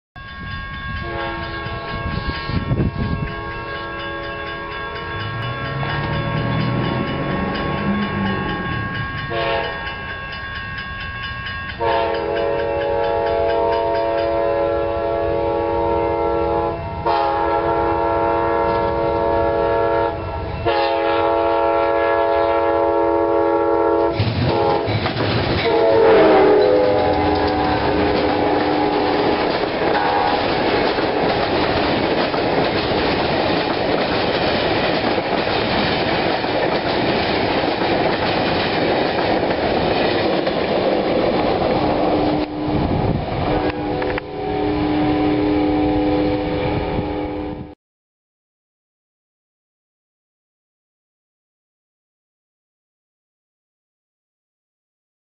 Amtrak #92 The NB Silver Star (inbound at Plant City FL 3-8-09
Amtrak PO92 The NB Silver Star. I wanted to try something different so I did a low angle shot on this one.